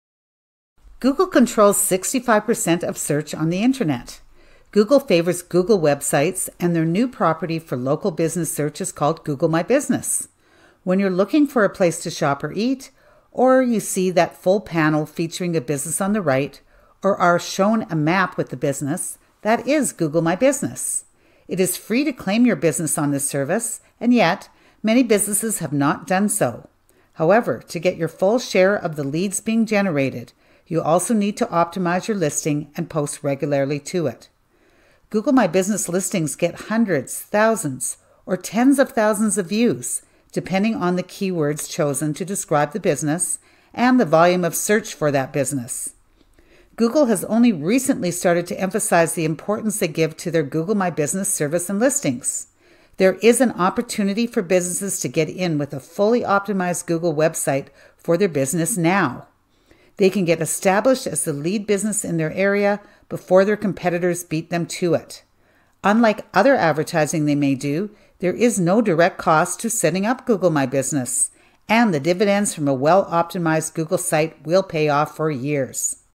Generate Leads for Local Calgary Businesses
Use a well-optimized google my business website to generate leads for local businesses in Calgary